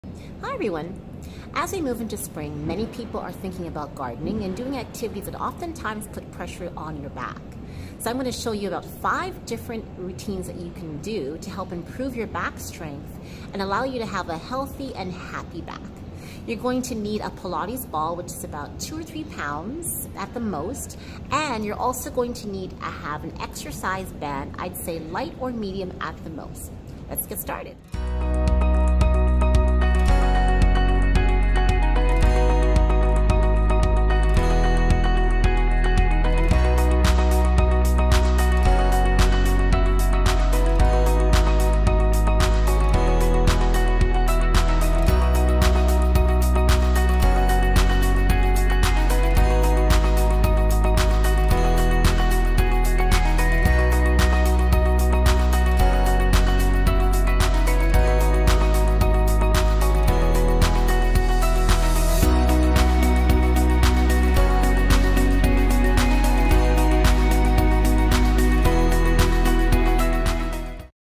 6 WEEKS TO YOUR BEST BACK EVER!
I’VE GOT YOUR BACK! LITERALLY 😉

With Spring just around the corner, many of you are planning to do gardening, de-cluttering, cycling and other activities that require a strong back! 💪🏾

Try these 5 exercises 3X per week, reps of 15-20 each for the next 6 weeks.

1) Back Extensions
2) Supers w/ Lat Pulldown
3) Swimmers
4) Supers w/ Band

5) Bird Dogs w/ Shoulder Rotation